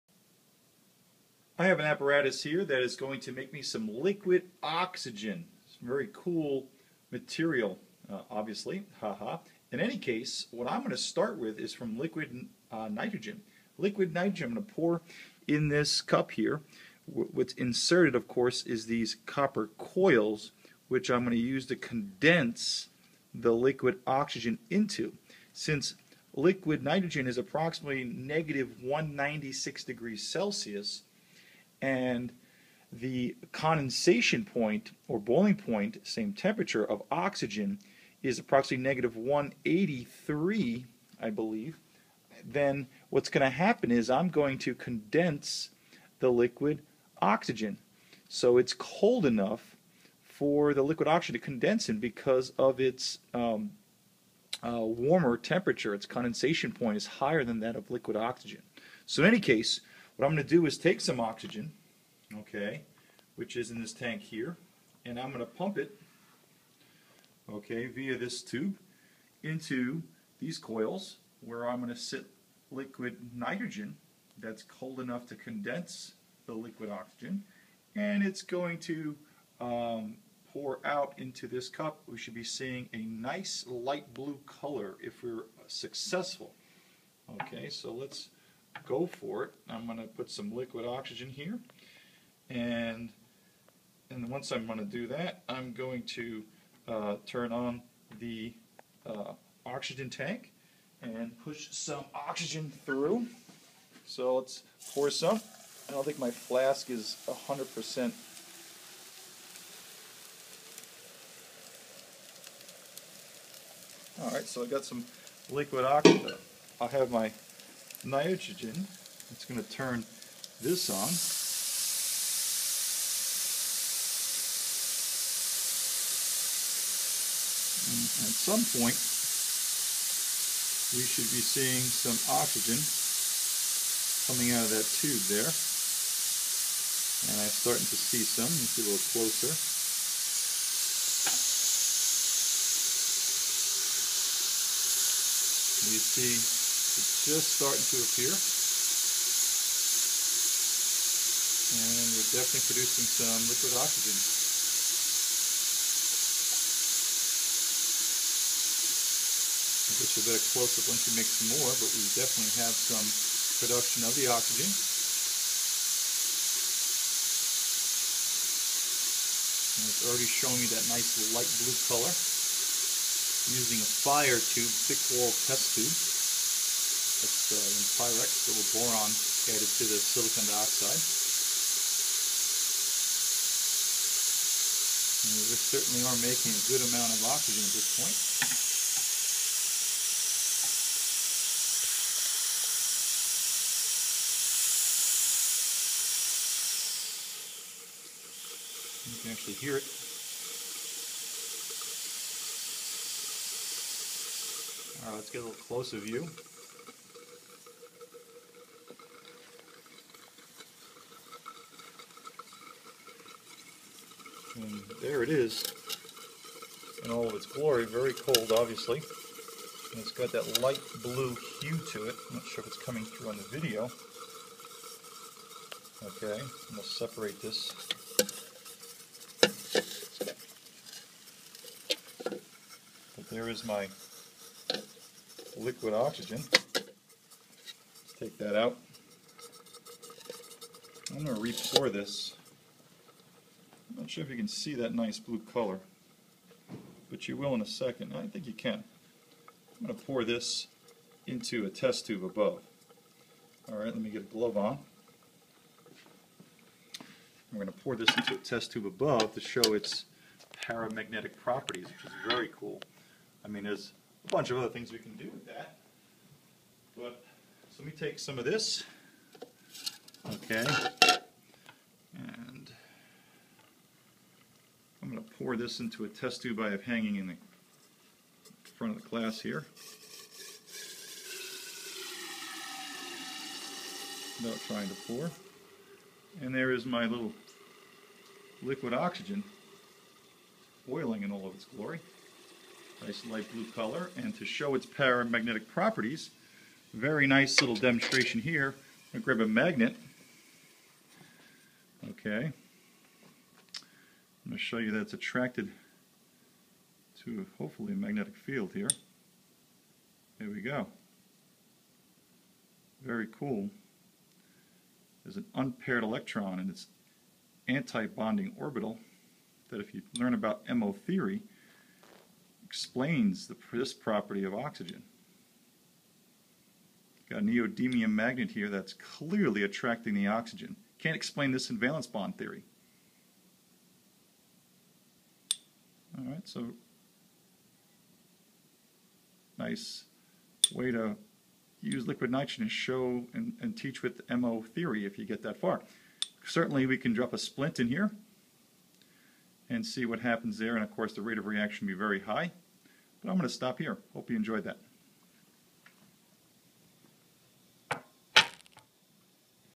LIquid Oxygen Preparation and Para magnetic Properties
Liquid nitrogen is used to condense oxygen gas into a liquid because of oxygen' warmer condensation or boiling point. This blue liquid's para magnetic properties are displayed as it attracts to a neodymium magnet. A very cool demonstration.